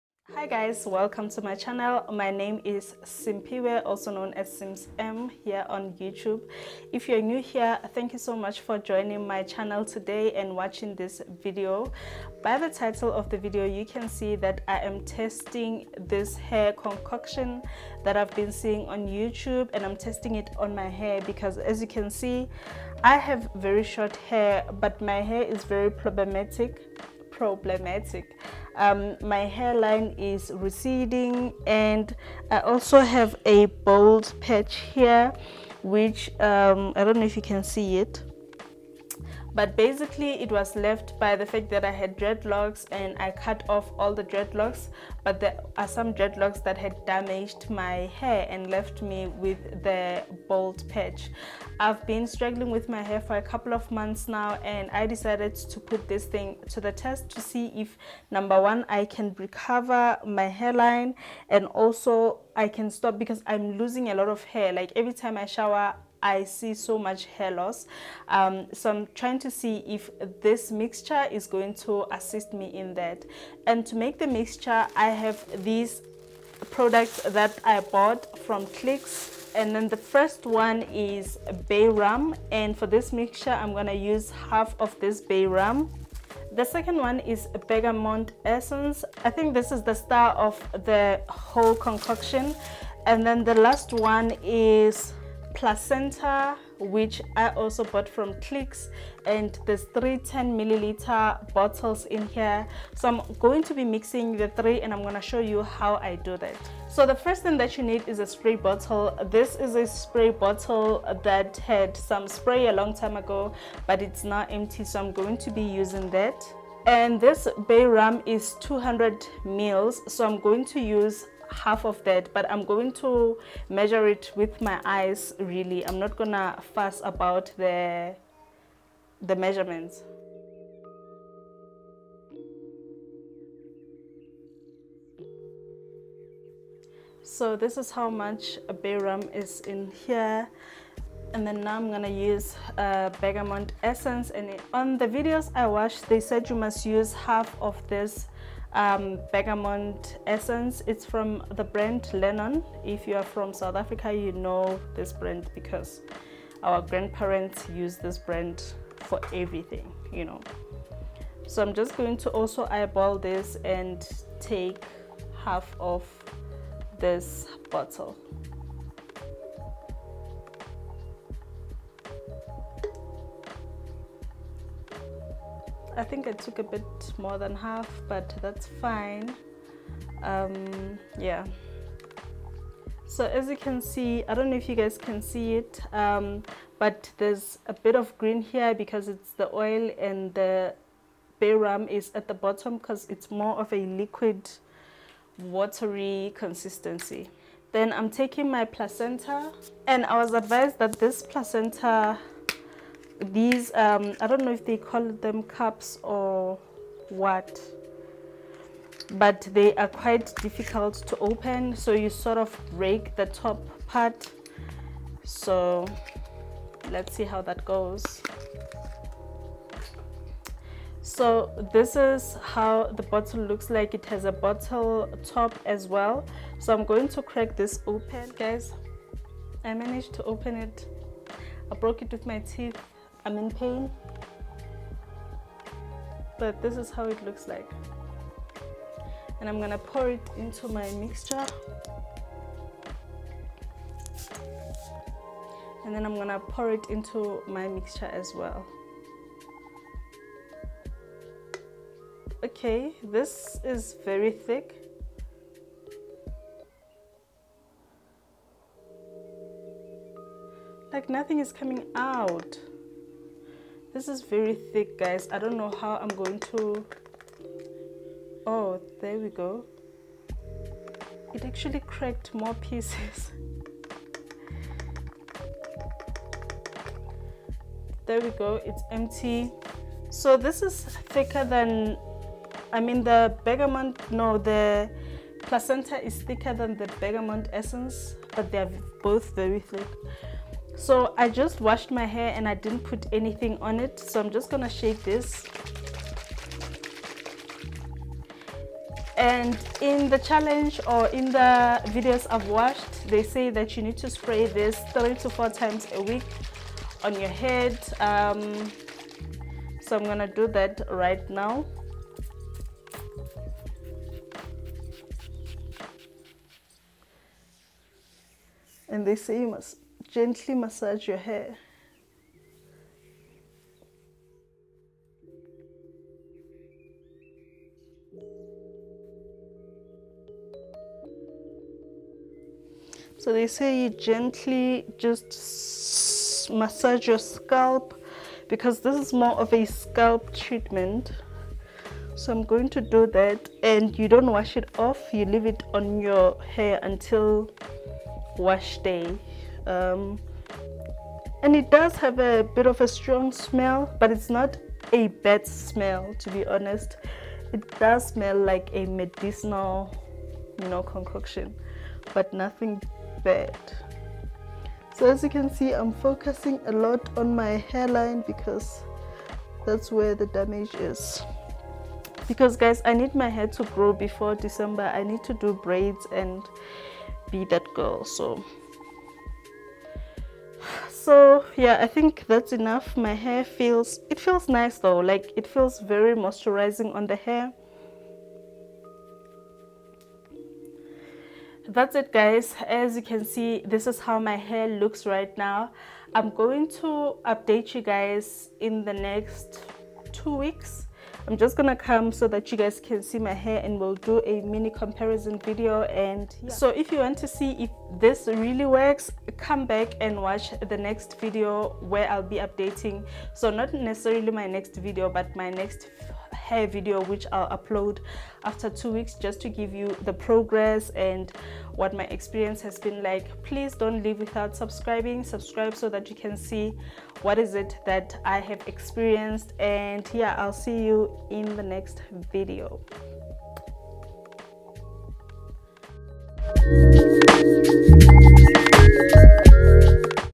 Does Bergamont Essence really work for Hair Growth? | My hairline is receding | Hair home remedies
Hi loves

I did a two weeks hair treatment using Bergamont Essence, Placenta and Bay rum. I will share my results in the next video.